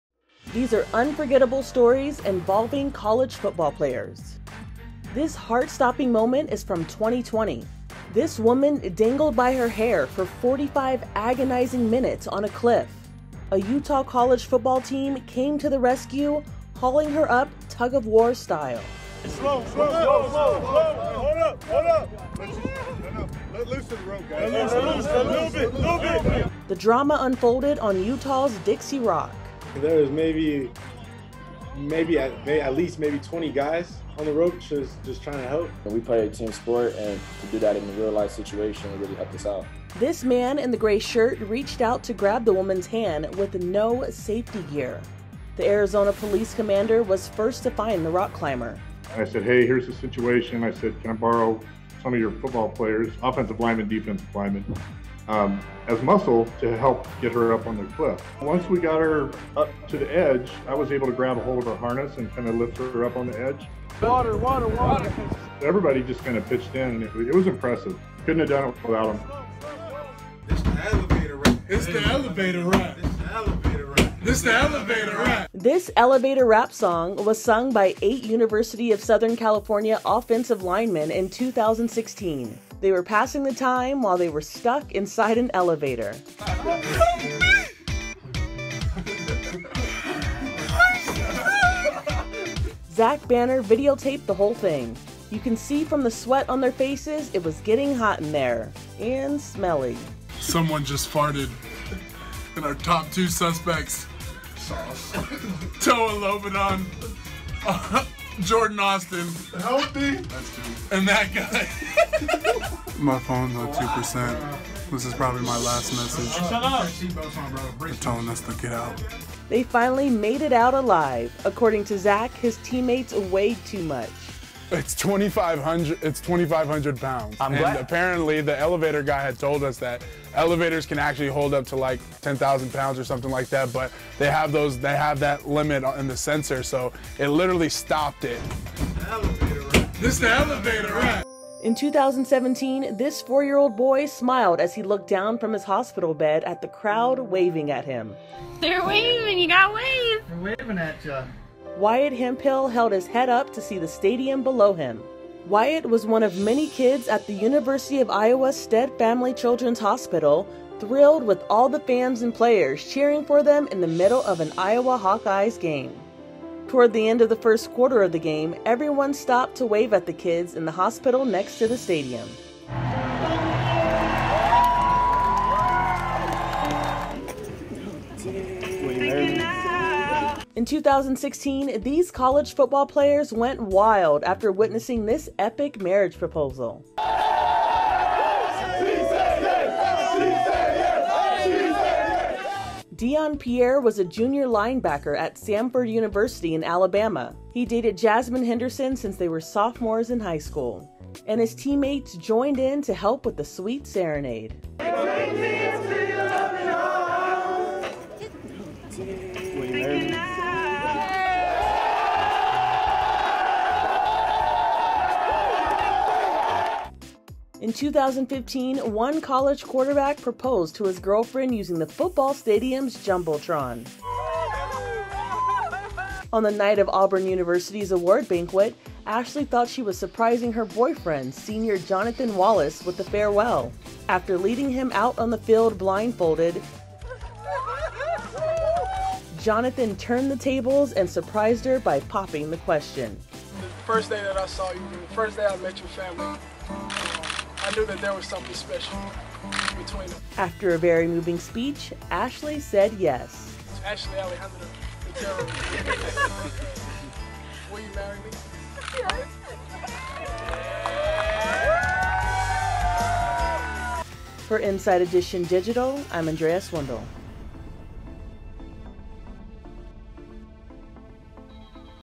Our Most Surprising College Football Stories
These are unforgettable stories involving college football players. In 2020, a woman dangled by her hair for 45 agonizing minutes from a cliff, and Utah college football players saved her. In 2016, University of Southern California football players got stuck in an elevator and creatively passed the time. In 2015 and 2016, two college football teams helped their teammates pull off epic proposals. Inside Edition Digital’s Andrea Swindall has more.